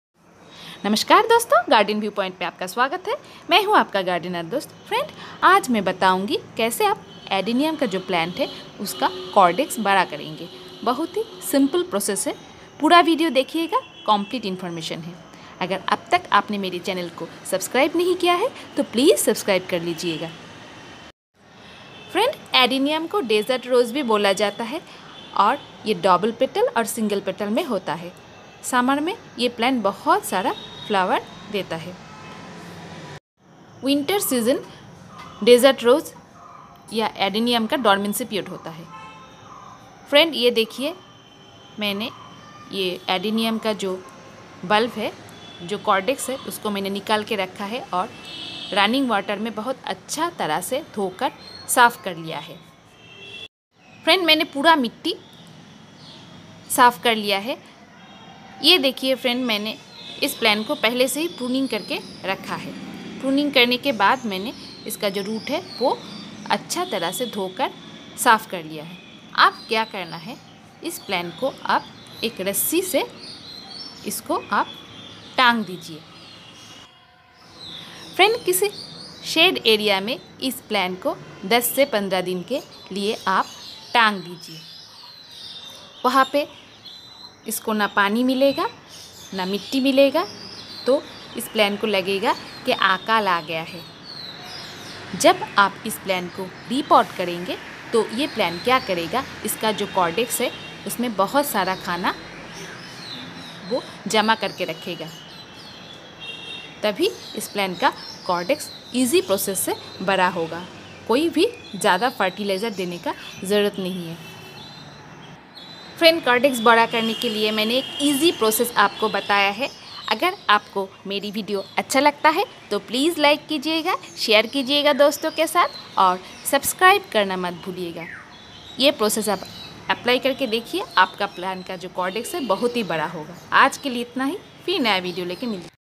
Adenium big Caudex in one month Easily/How to make adenium caudex big/ Adenium plant care tips
How to make big adenium caudex at home easily,
Get a fat adenium caudex in one month,
Adenium plant care tips,